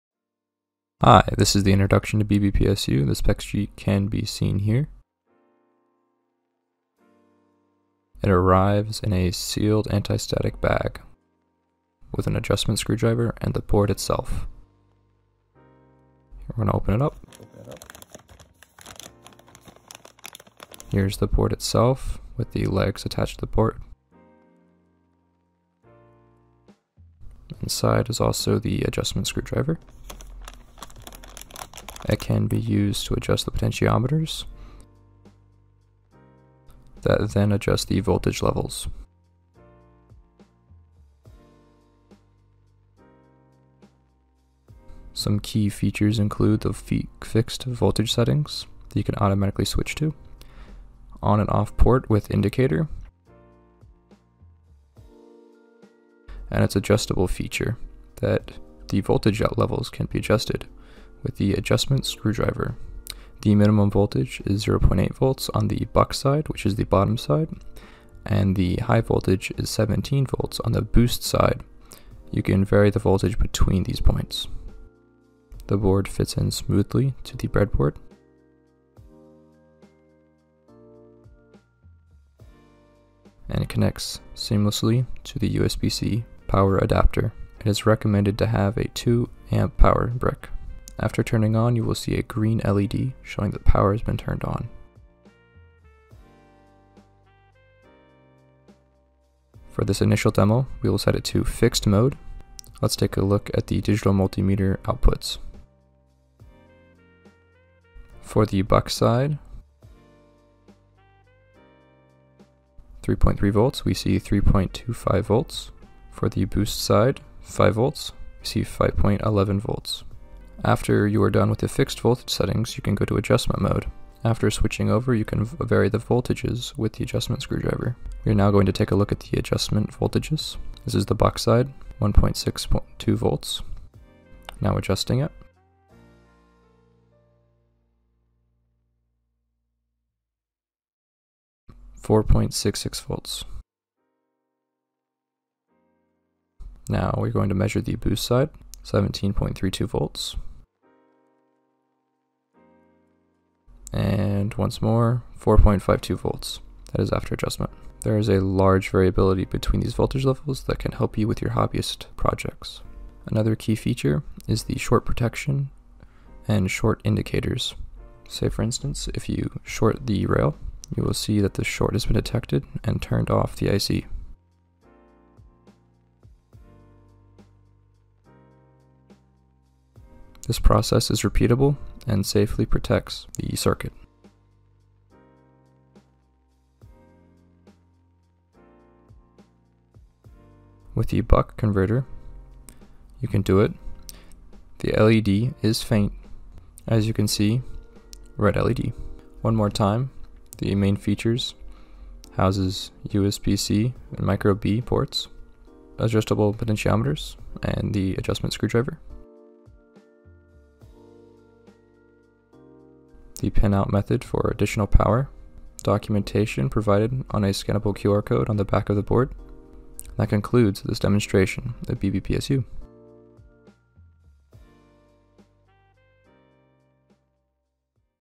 BBPSU Demonstration Video | Advanced Breadboard Power Supply For DIY Projects
BBPSU - Bread Board Power Supply Unit

·         Small 20x54mm design

·         Compatible with the breadboard (MB-102+)

·         Adjustable buck and boost converters (0.8V-17V)

·         Fixed voltage settings (3.3V/5V)

·         Easy settings configured with switches

·         USB C and USB Micro B Compatible

·         Isolated overcurrent protection